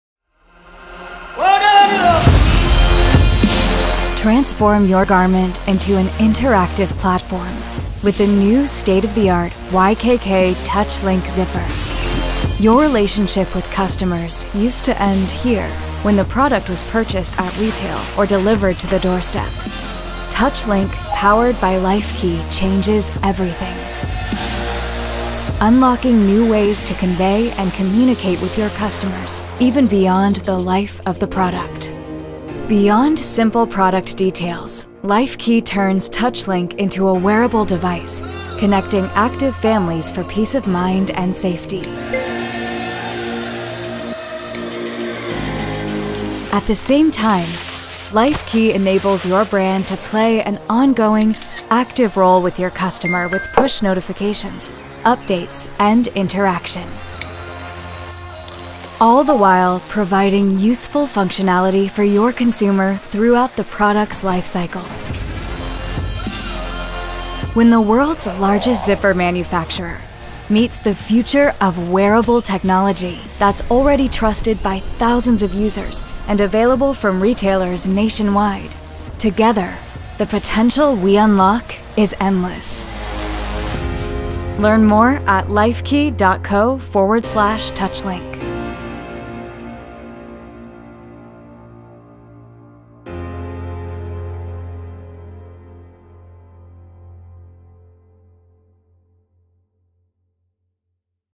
Lifekey x YKK® TouchLink™ Smart Zipper
Introducing YKK® TouchLink™, a smart zipper powered by Lifekey. Lifekey's operating system, Tap™ OS, unlocks endless potential for manufacturers, brands, and consumers alike. Want to see the future?